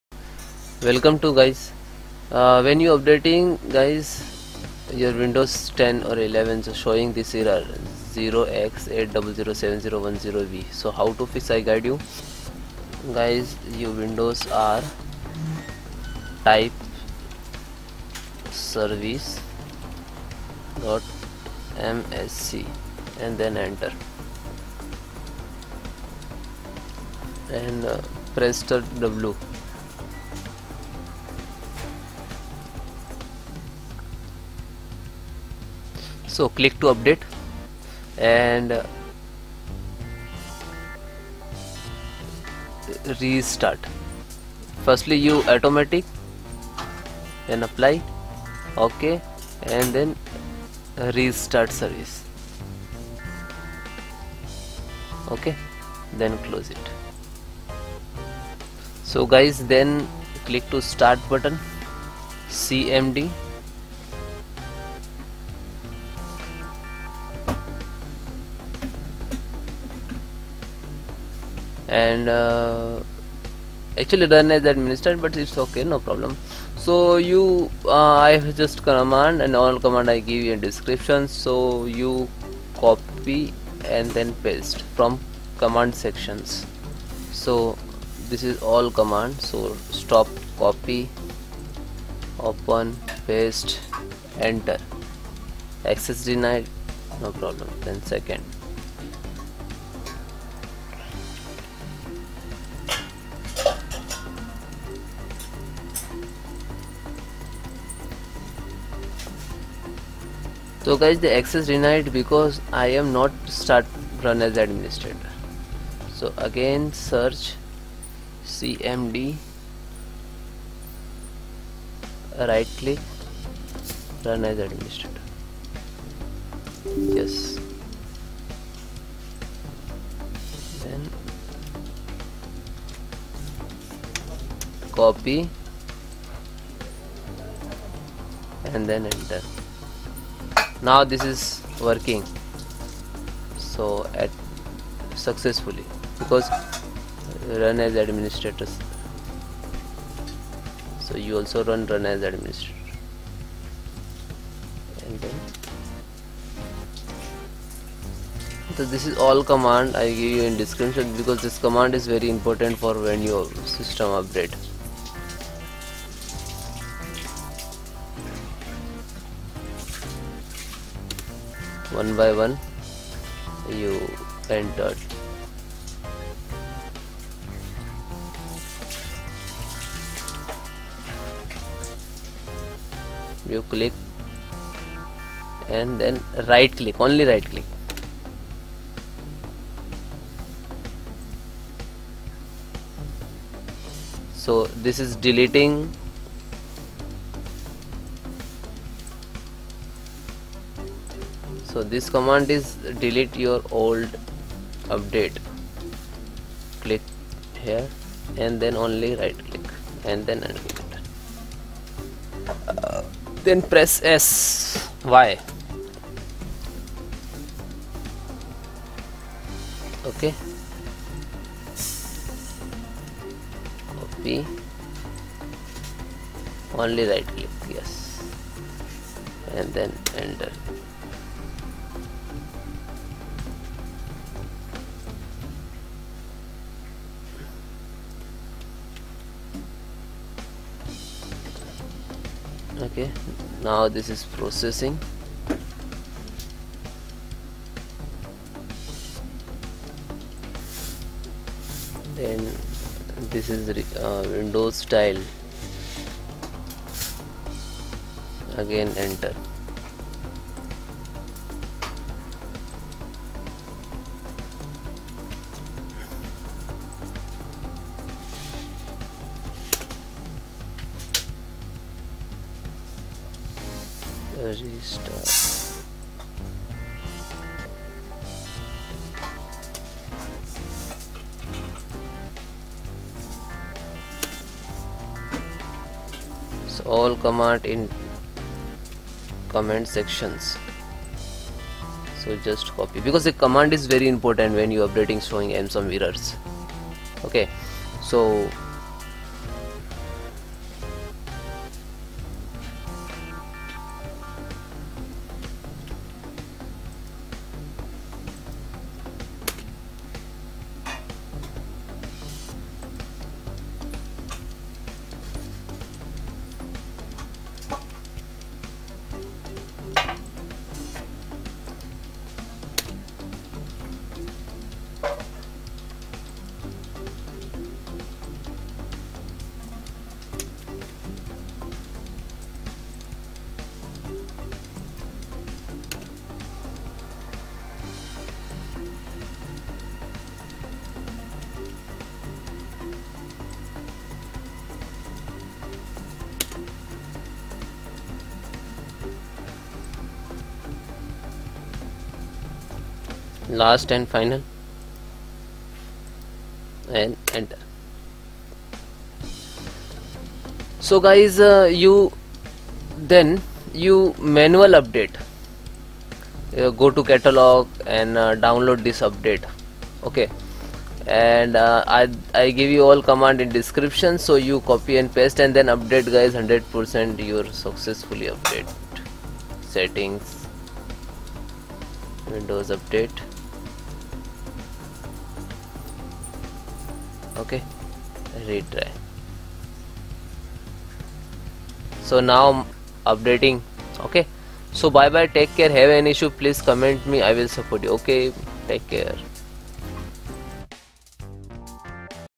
How to fix Windows update error 0x8007010b windows 10 or 11
How to fix Windows update error 0x8007010b windows 10

Open Command Prompt as administrator again
Copy and paste the following commands, pressing Enter after each: